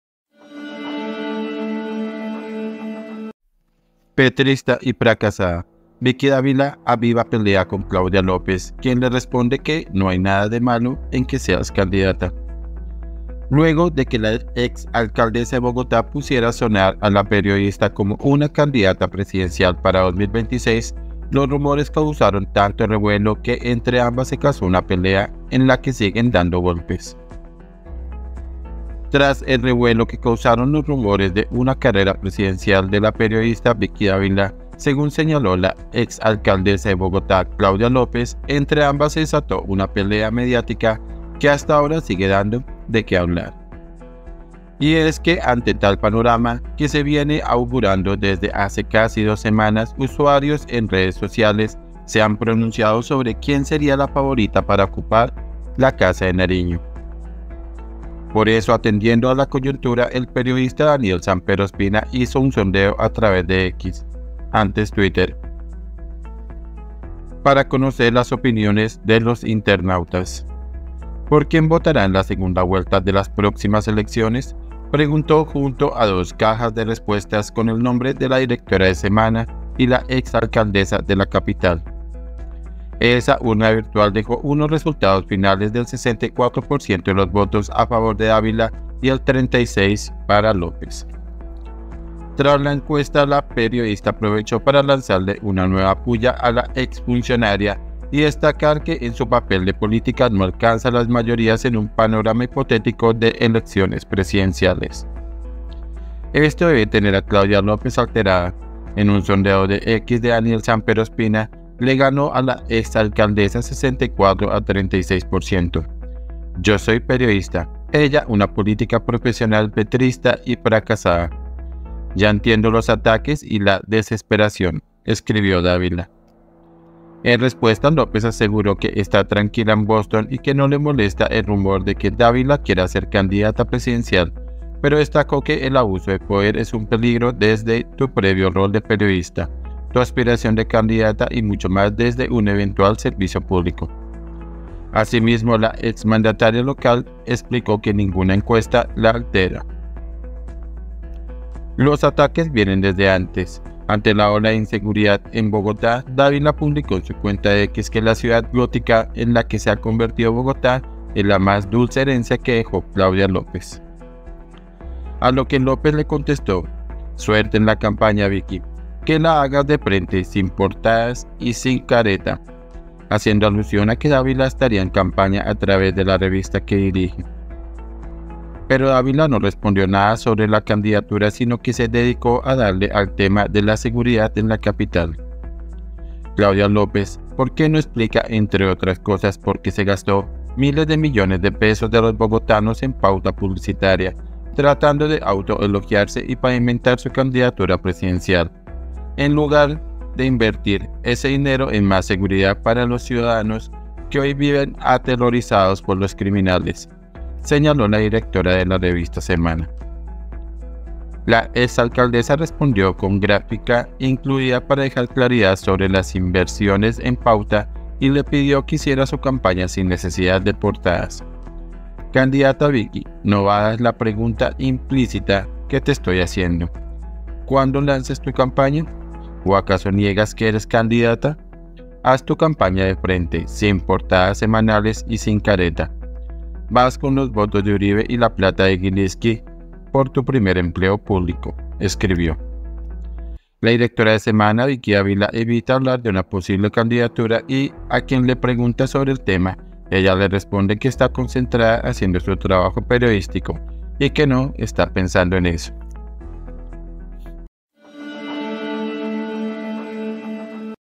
'PETRISTA Y FRACASADA' 🔴 Vicky Dávila aviva pelea con Claudia López
Luego de que la exalcaldesa de Bogotá pusiera a sonar a la periodista como una candidata presidencial para 2026, los rumores causaron tanto revuelo que entre ambas se cazó una pelea en la que siguen dando golpes.
EL COLOMBIANO